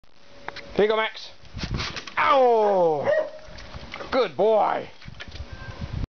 Max eats Grasshopper
Max loves to eat grasshoppers, we have been invaded by them so he has been busy.  Hard to catch on tape unless you do it this way.  Almost cost me a dog bite.